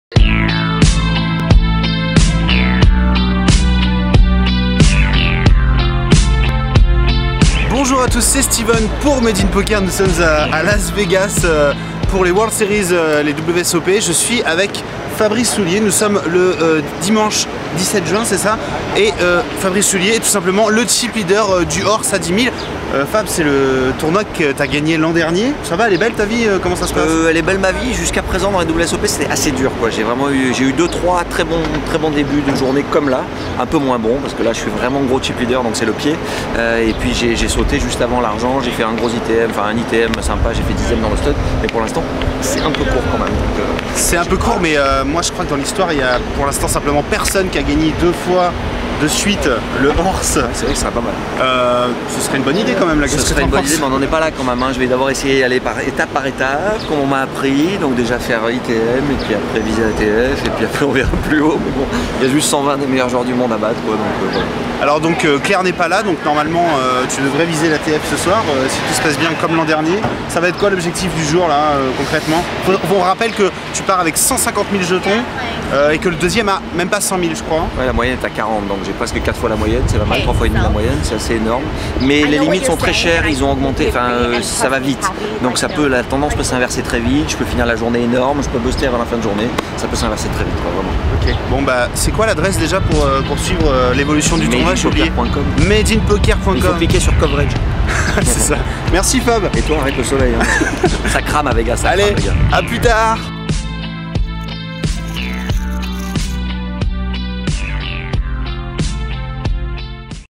Fabsoul, chipleader du HORSE à 10 000$
Fabrice "Fabsoul" Soulier se confie quelques minutes avant de repartir sur le H.O.R.S.E à 10 000$ en position de chipleader ! Un tournoi qu'il a remporté en 2011 !